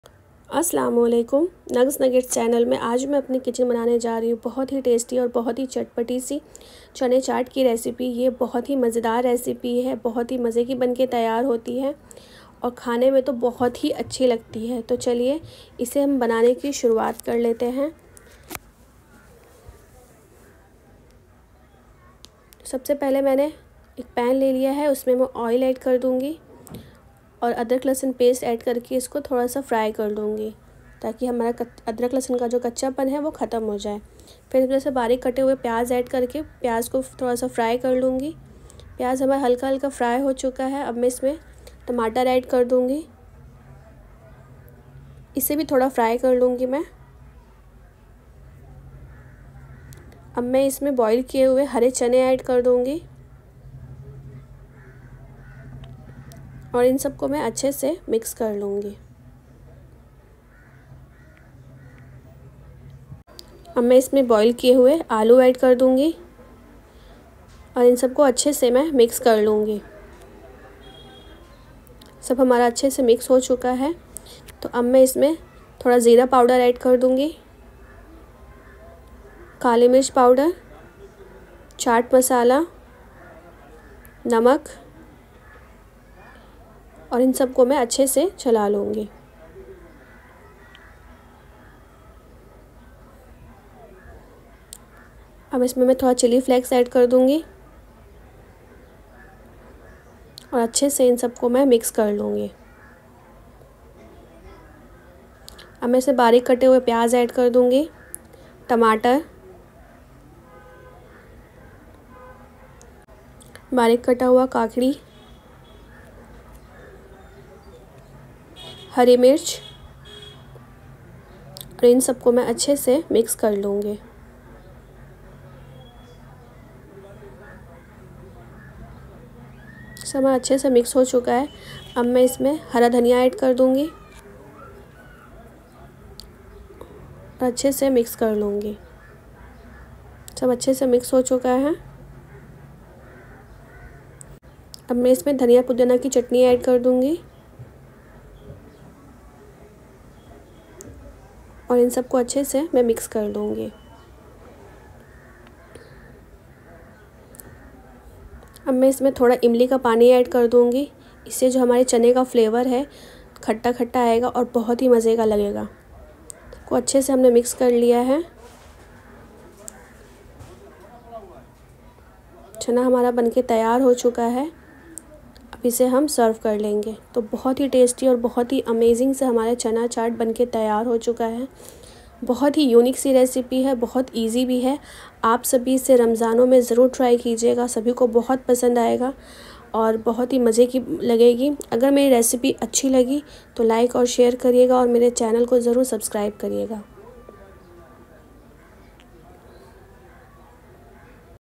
Chatpata chana chaat recipe || Hare chane chaat ki unique recipe 🤤😋ramzanspecial
Ingredients
2 tbsp oil
2 tspn ginger/garlic paste
3 onion
2 tomatoes
2 cup boiled green chana
3 boiled potatoes
1 tspn cumin powder
1 tspn pepper powder
2 tspn chaat masala
Salt according to taste
2 tspn chilli flakes
1 cucumber
2/3 green chillies
Coriander leaves
1 tbsp green chutney
2 tbsp tamarind water.